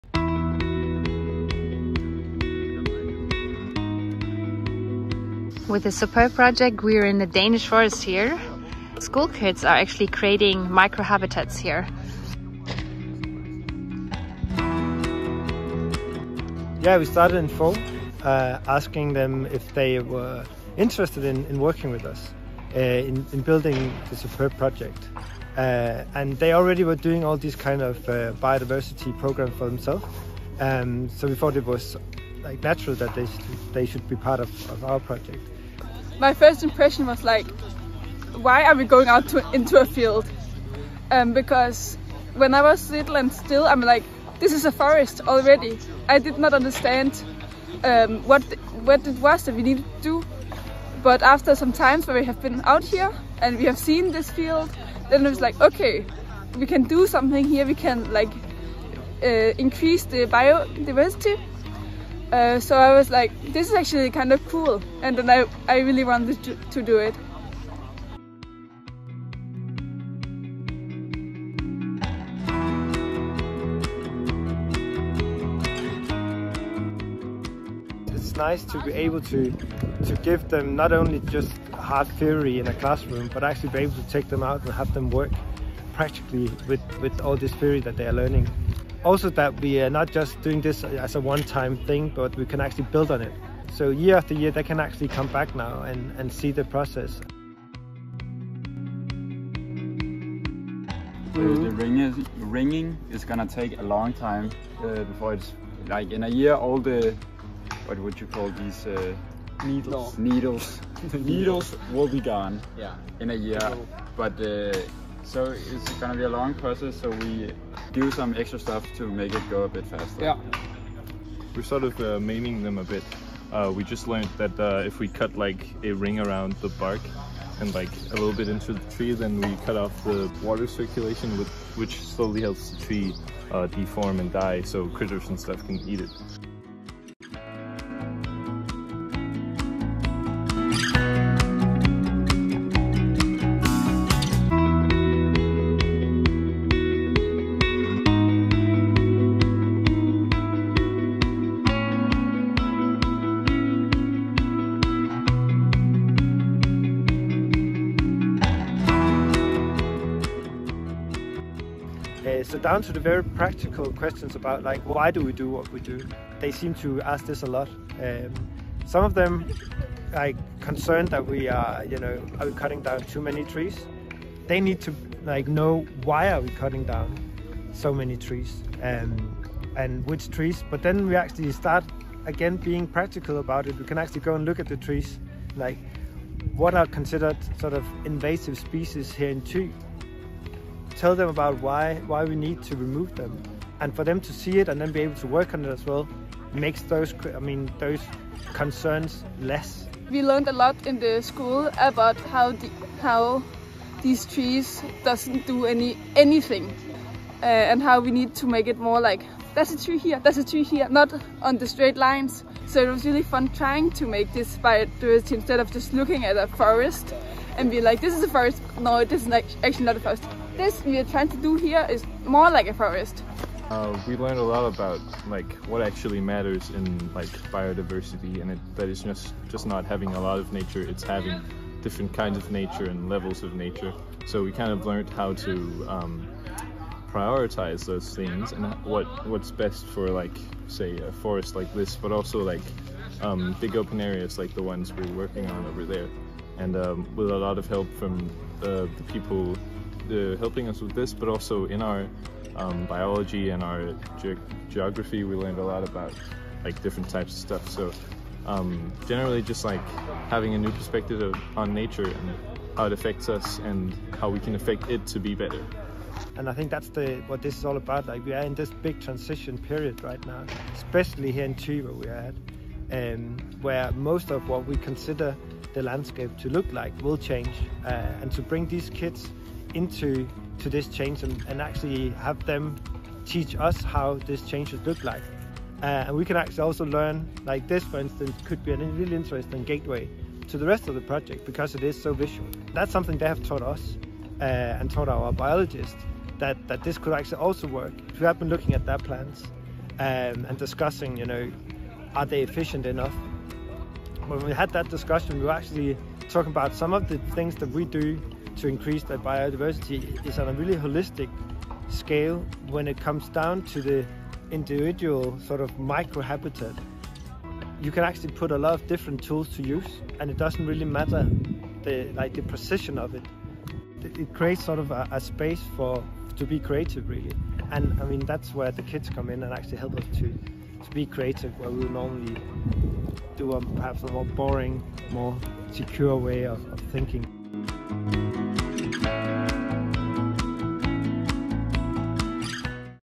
SUPERB Danish demo site
Our Danish demo in Thy invited students to get hands-on experience on the field & learn more in-depth knowledge on the important role of biodiversity. We also learned how students can contribute to this long process, curious how?